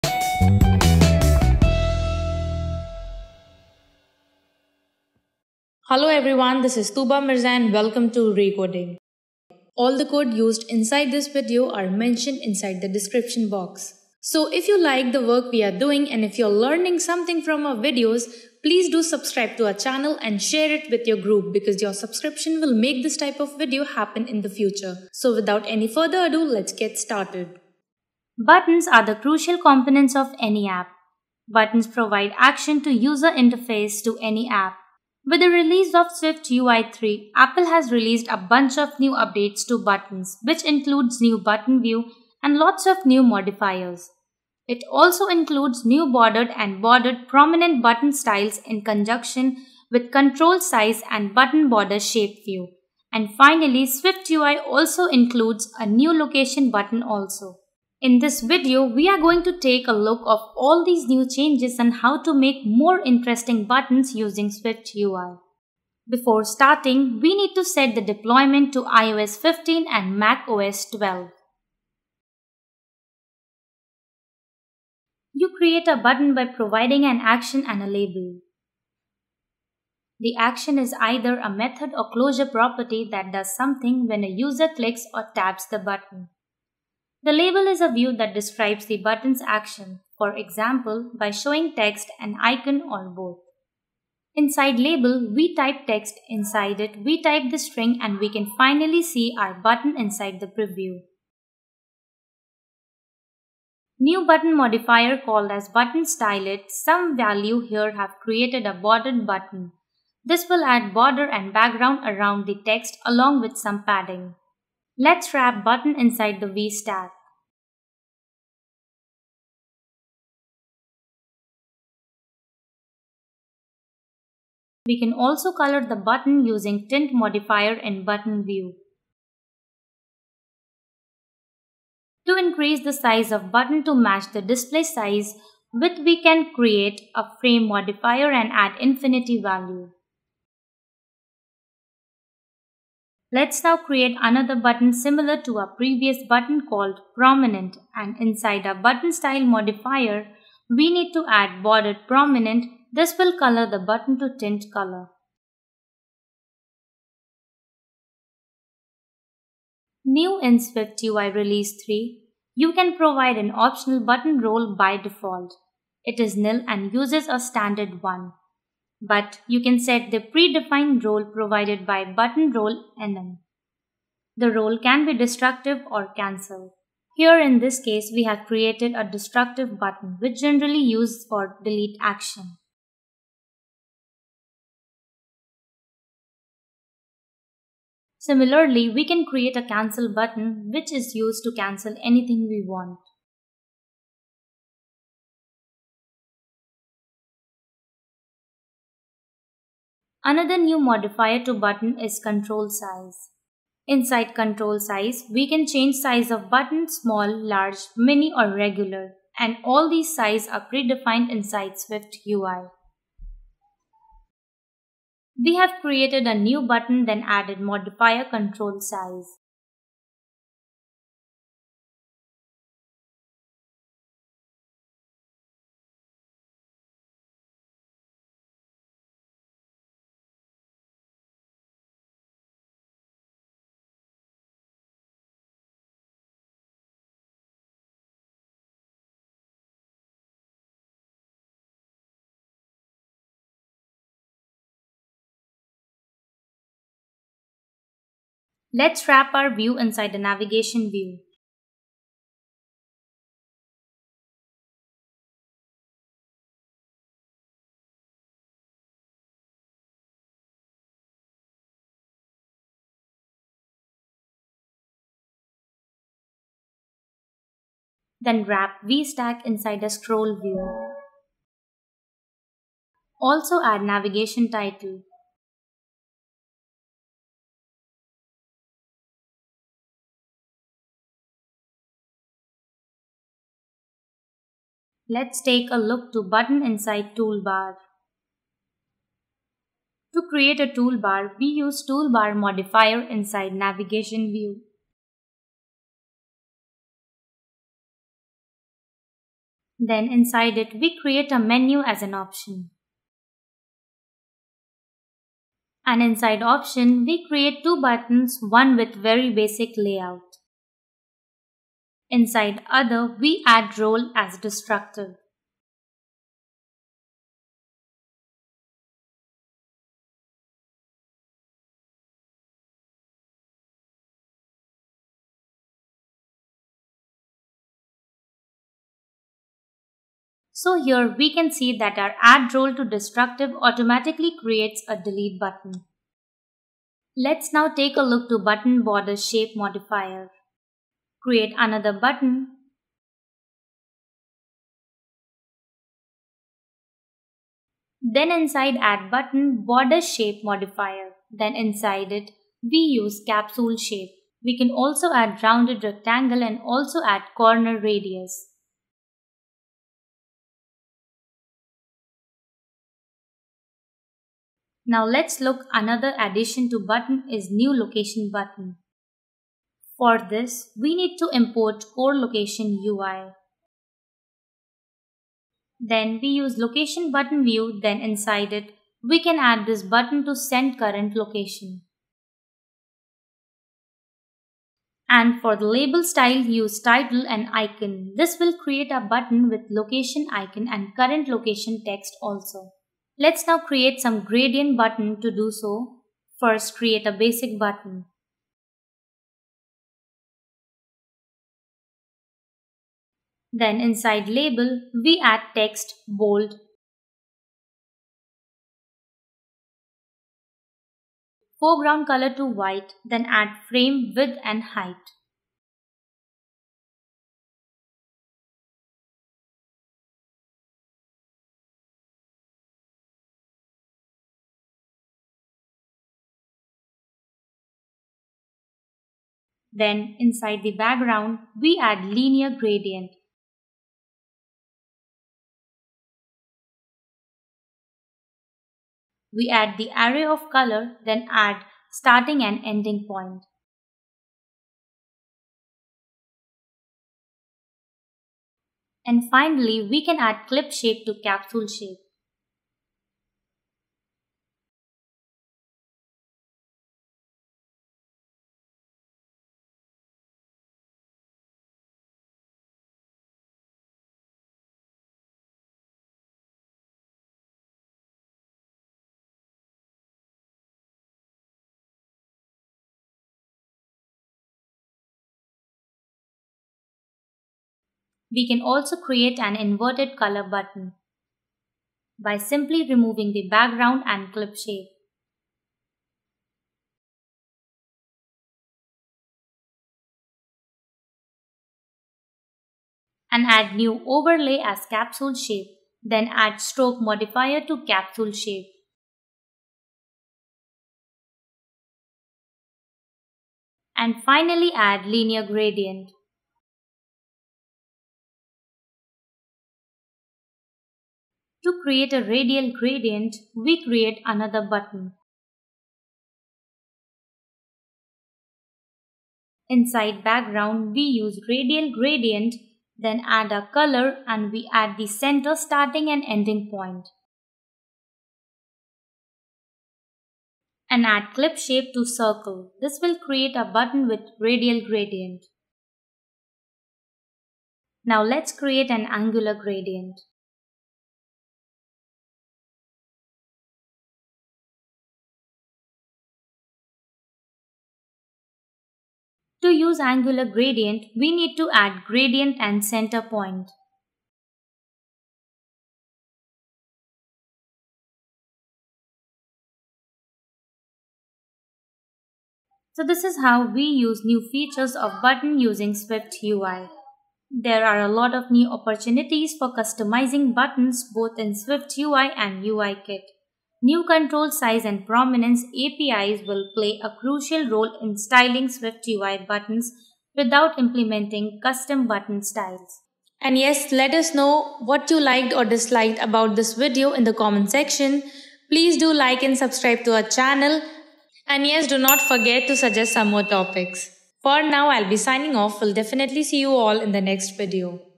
New Button Customization Styles In SwiftUI 3.0
Buttons are the crucial components of any app. Button Provide action to User Interface to any app. With the release of SwiftUI 3, Apple has released a bunch of new updates to Buttons which includes new Button View and lots of new modifiers, it also includes new bordered and bordered prominent button styles in conjunction with control size and Button Border shape view and finally SwiftUI also include a new Location Button also

In this video we gonna take a look all these new changes and how to make more interesting button using SwiftUI 3.

Timeline
Intro (00:00)
Creating Xcode Project (00:27)